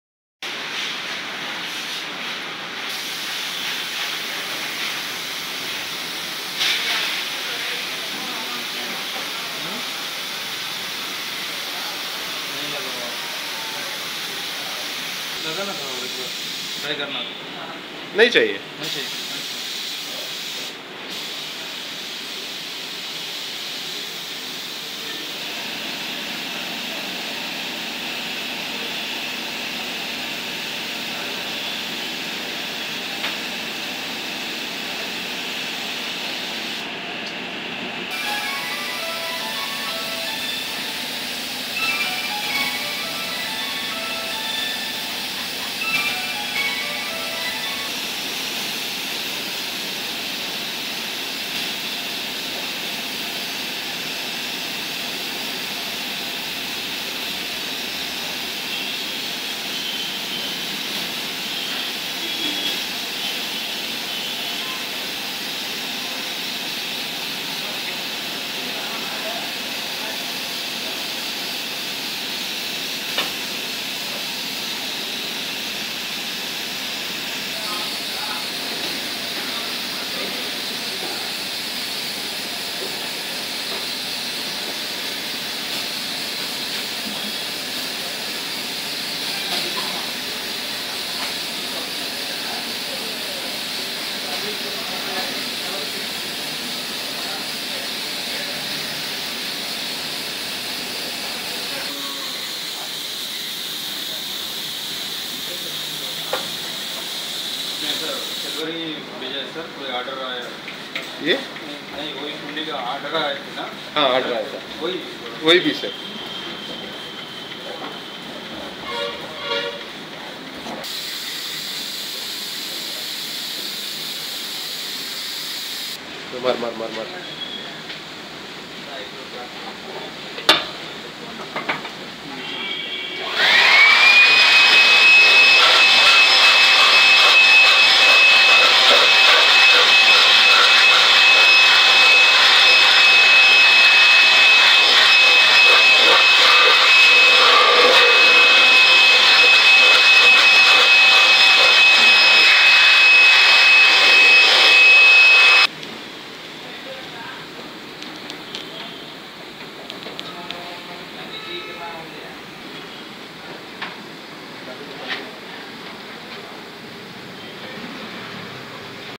How to chrome mud flower pots
Eazy way to chrome mud flow pots... you can chrome on any surface.